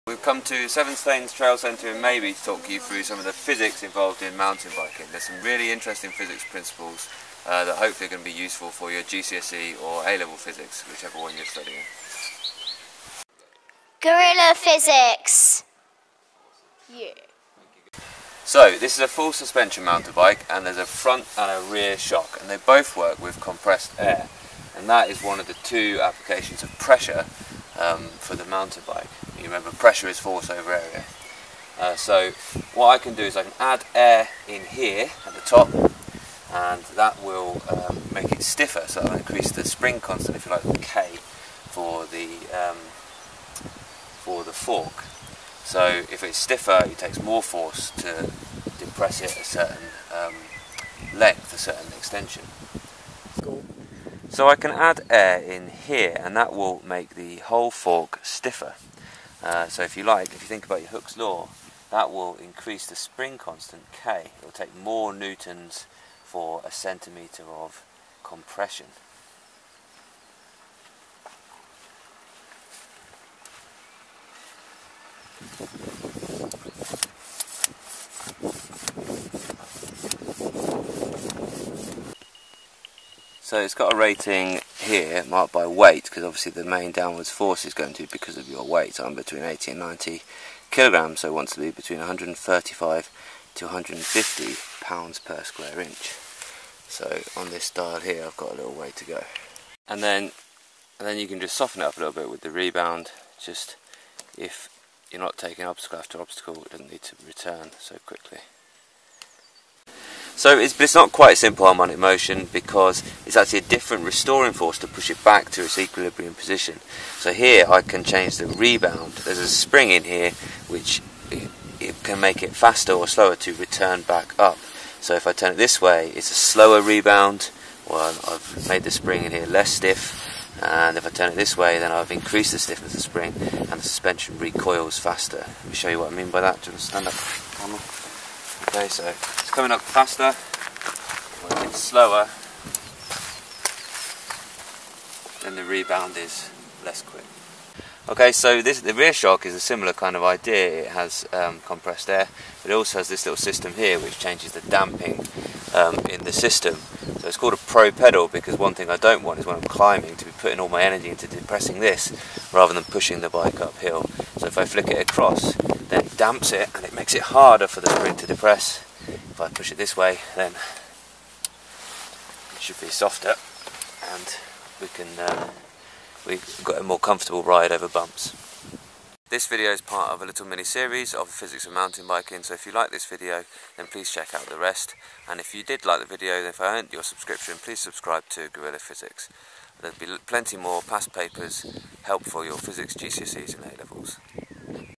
Mountain Bike Physics [1/4] - Suspension - GCSE and A Level Physics
Some really interesting applications of your GCSE and A Level Physics.  We've gone to the 7Stanes Trail Centre in Mabie to demonstrate how some of the physics you've been learning about is applied to mountain biking.  This video, suspension, which is an application of pressure, Hooke's Law and damping.